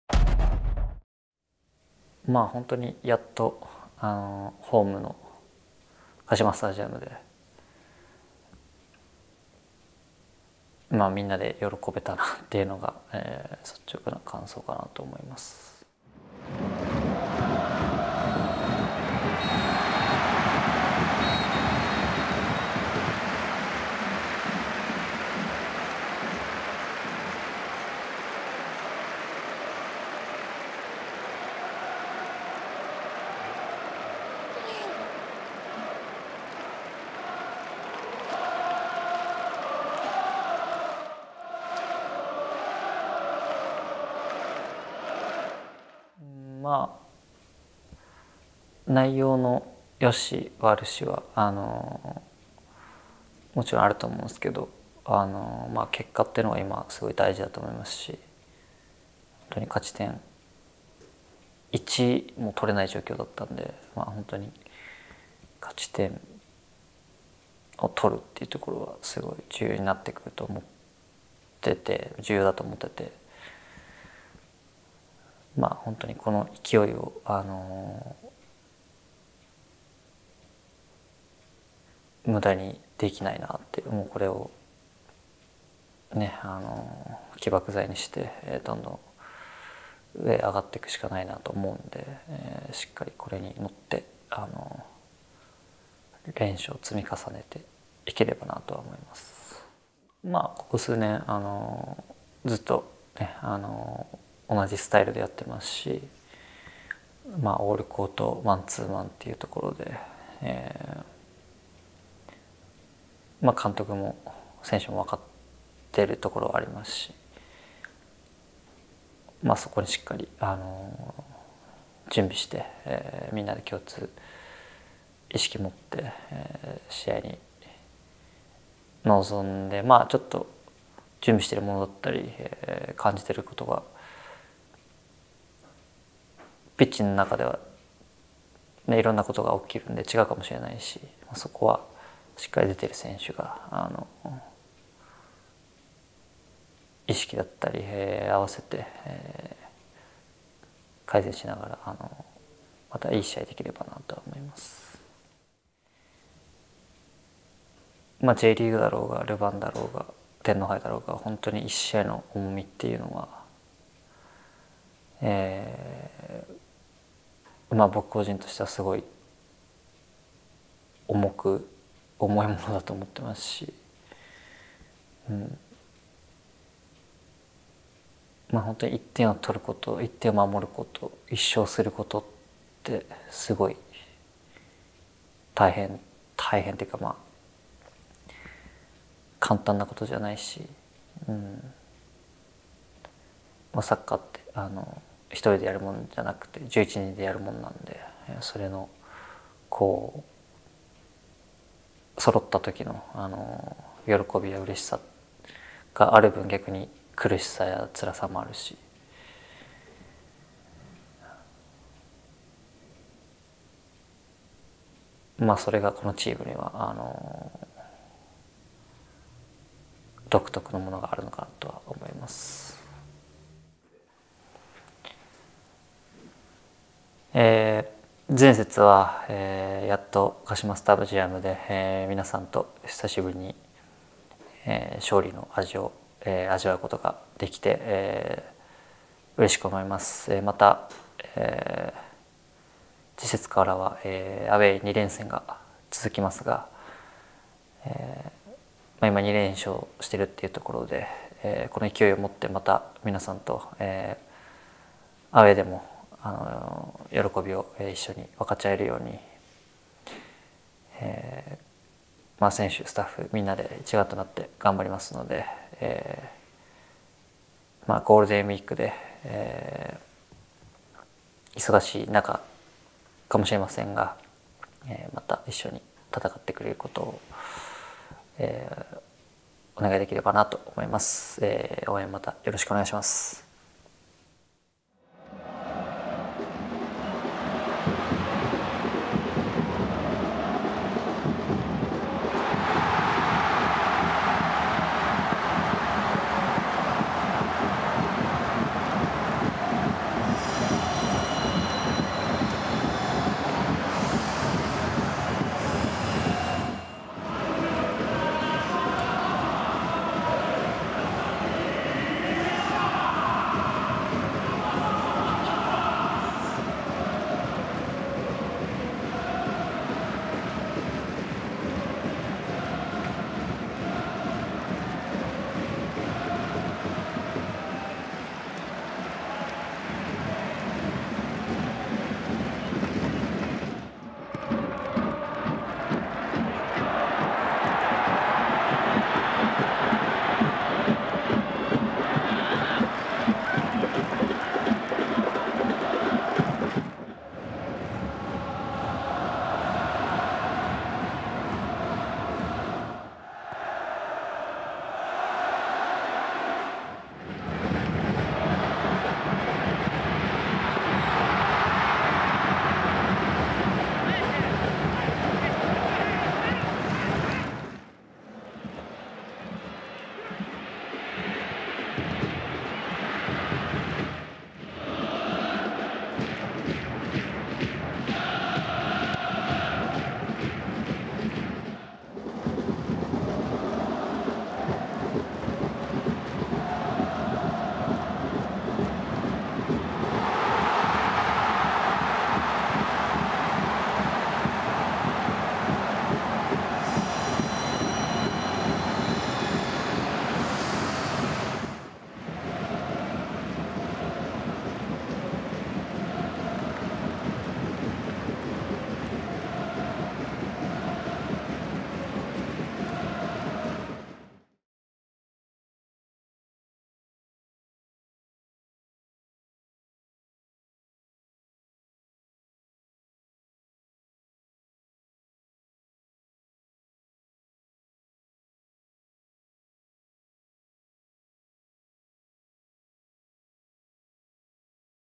【鹿島アントラーズ】PRE-MATCH INTERVIEW～土居 聖真～
5/3（水・祝）に行われる北海道コンサドーレ札幌戦に向けて、土居選手へのインタビューです。